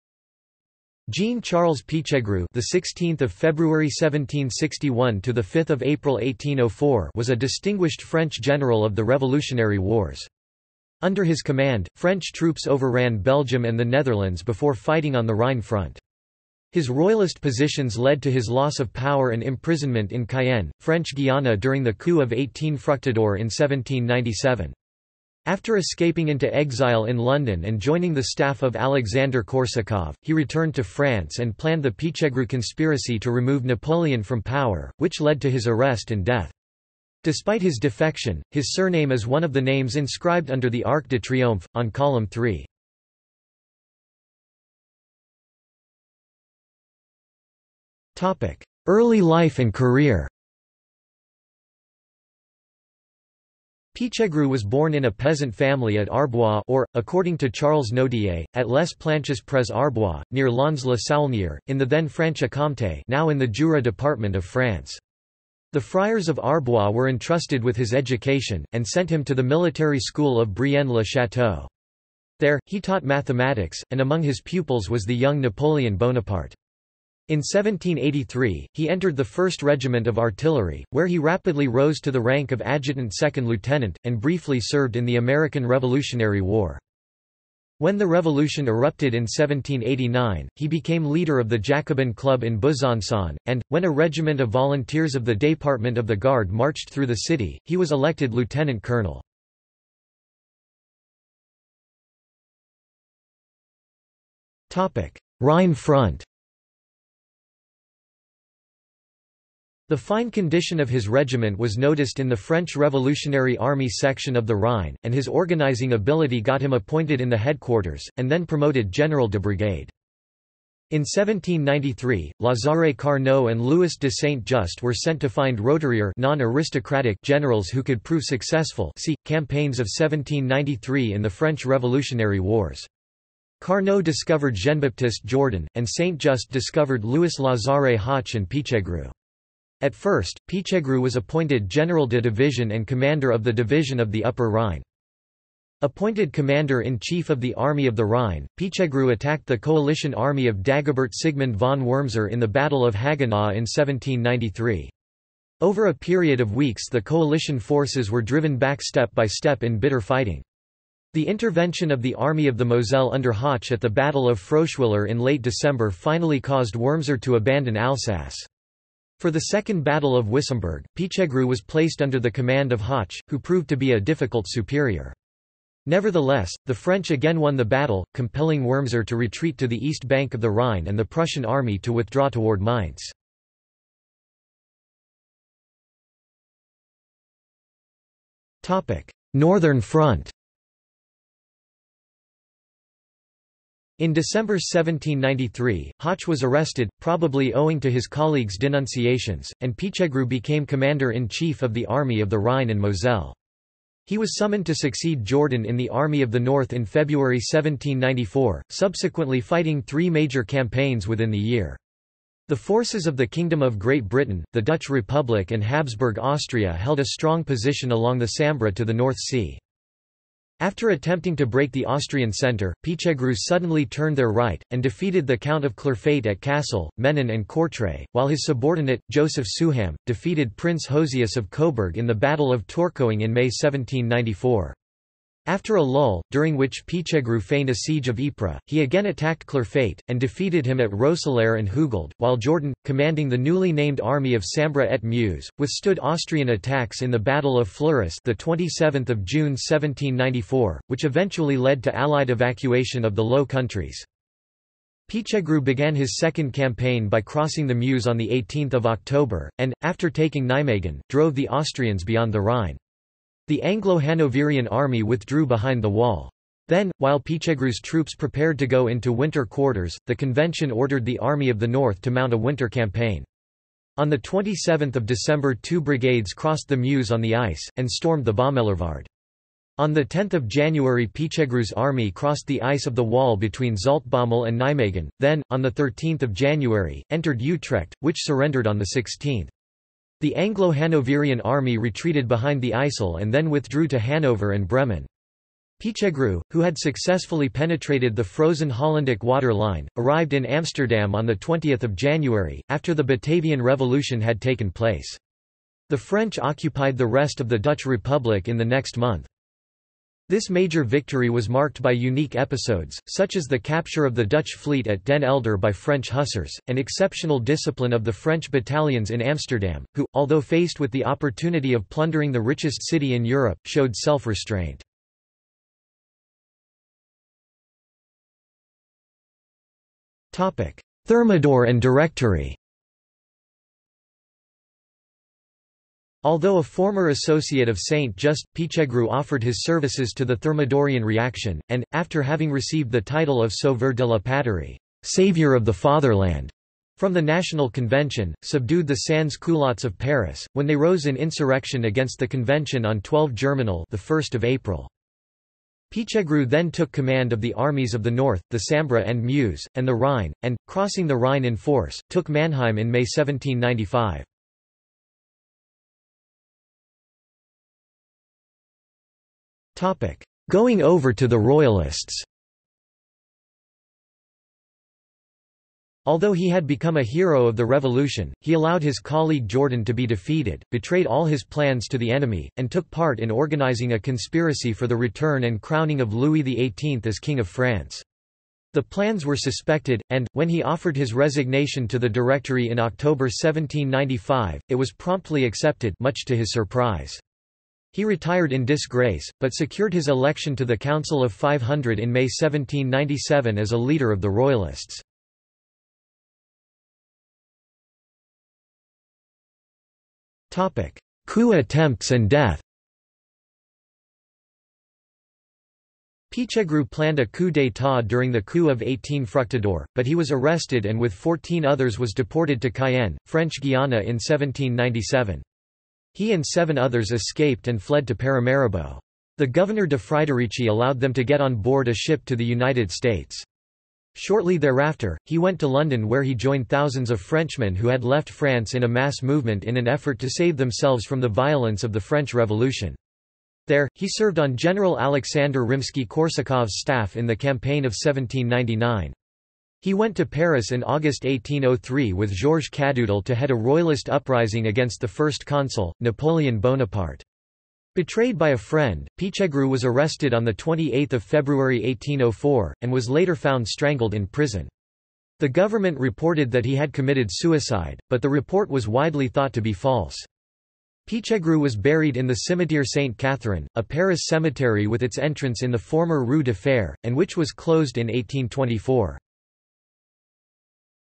Jean-Charles Pichegru | Wikipedia audio article
This is an audio version of the Wikipedia Article:
Jean-Charles Pichegru

00:00:44 1 Early life and career
00:01:41 2 Rhine front
00:03:05 3 Northern front
00:05:26 4 Thermidor and Directory
00:06:06 5 Going over to the Royalists
00:06:46 6 Coup attempts and death

- Socrates

SUMMARY
Jean-Charles Pichegru (16 February 1761 – 5 April 1804) was a distinguished French general of the Revolutionary Wars. Under his command, French troops overran Belgium and the Netherlands before fighting on the Rhine front. His royalist positions led to his loss of power and imprisonment in Cayenne, French Guiana during the Coup of 18 Fructidor in 1797. After escaping into exile in London and joining the staff of Alexander Korsakov, he returned to France and planned the Pichegru Conspiracy to remove Napoleon from power, which led to his arrest and death. Despite his defection, his surname is one of the names inscribed under the Arc de Triomphe, on Column 3.